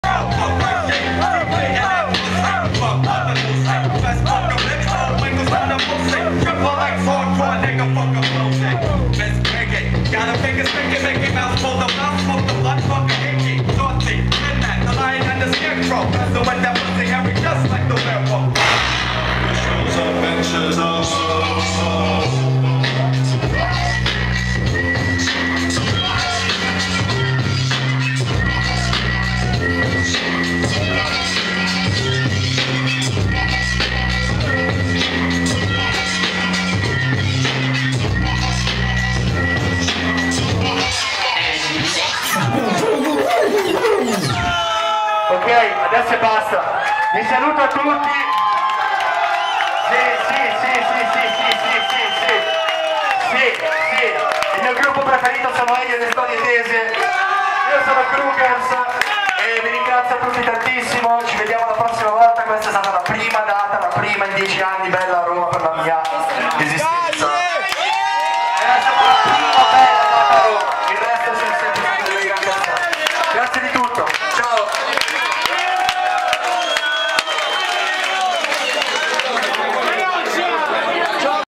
Crookers *Phra* Encore with Hip-Hop @ Adidas Stripes Sessions (23/10/12)
Phra from Crookers closes with some classic hip-hop he said he use to listen to when he was 12. At the Adidas store on Via Del Corso in Roma, Italy for the Adidas Stripes Sessions show.